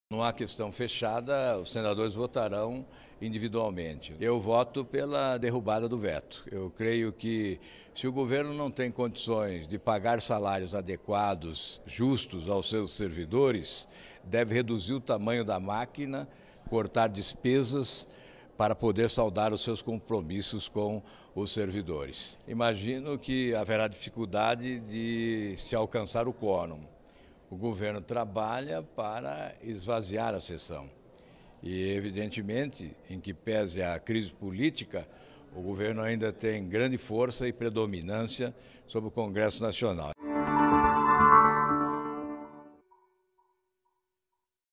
Senador Alvaro Dias do PSDB-PR
Senador Alvaro Dias vota pela derrubada do veto 26/2015. Se o governo não consegue pagar os trabalhadores deve enxugar a máquina.